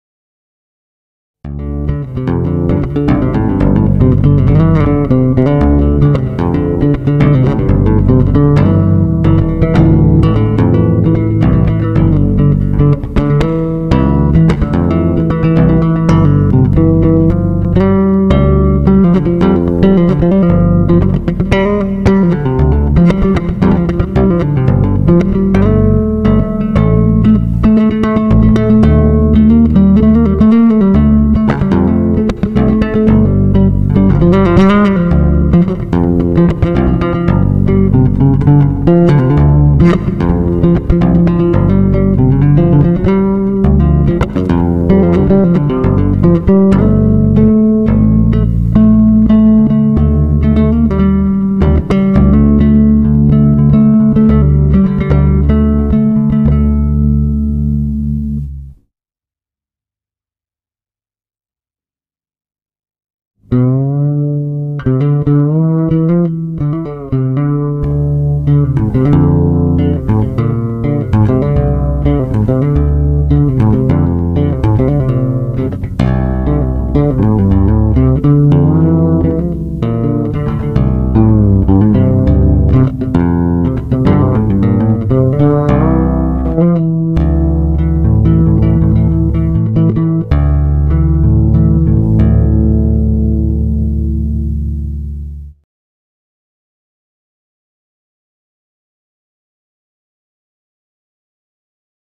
Rotator bas håndbygget af Bjørn Rønnow
Bjørn Rønnow har designet og håndbygget denne super spændende "Rotator" bas, som  han giver en smagsprøve på i videoen.
Bjørn har startet firmaet Nordic Guitars med en vision om at skabe et nydesignet bas instrument med mange muligheder for musikalsk udtryk. På videoen demonstrerer han den færdige Rotator model som han har arbejdet og videreudviklet på igennem adskellige år.